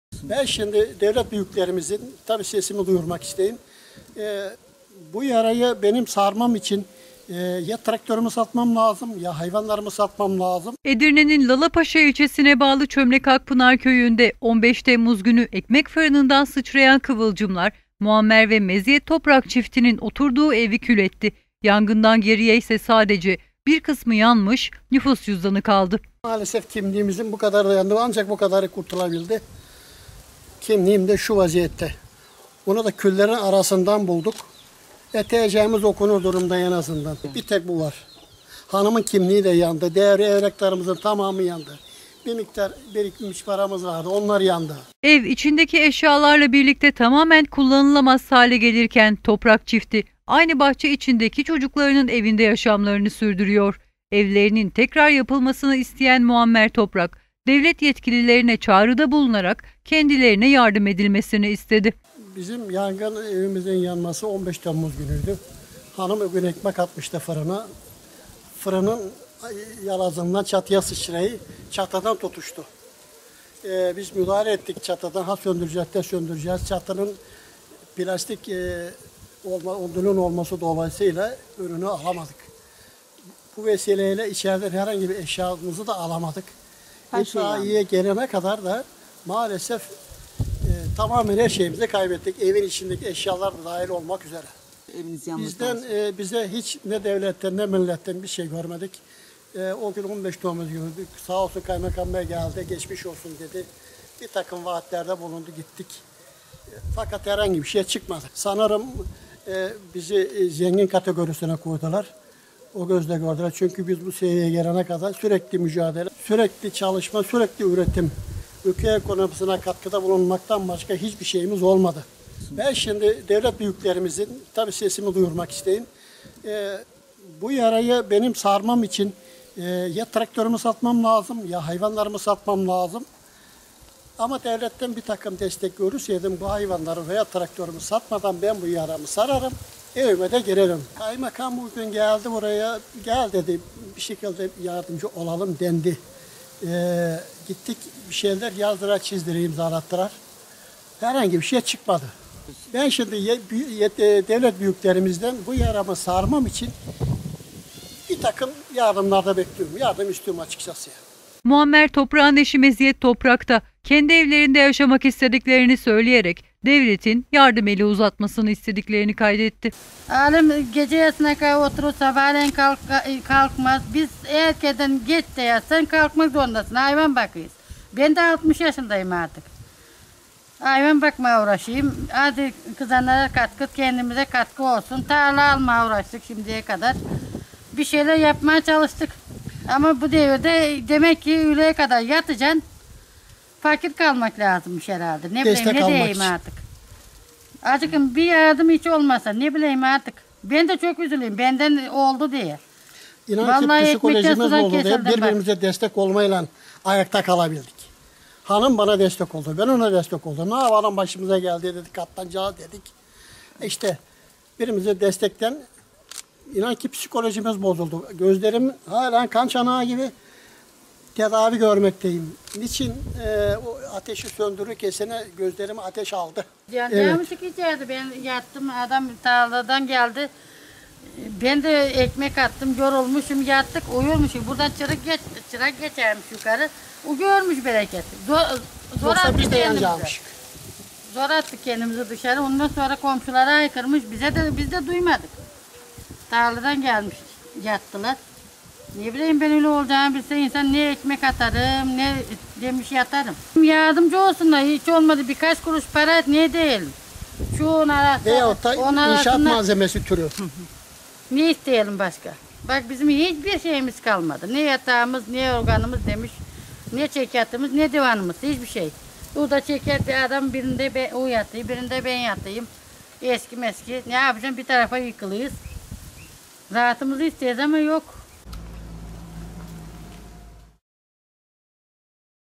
EVİ YANAN TOPRAK AİLESİ DEVLETTEN YARDIM BEKLİYOR
Edirne’nin Çömlekakpınar Köyü’nde 15 Temmuz günü evi yanan Toprak ailesi, devletin yardım elini uzatmasını bekliyor. Yangında sadece nüfus cüzdanını kurtaran Muammer Toprak, “yanan evimi onarmam için hayvanlarımı ya da traktörümü satmam lazım. Devlet büyüklerinden yardım bekliyorum” dedi.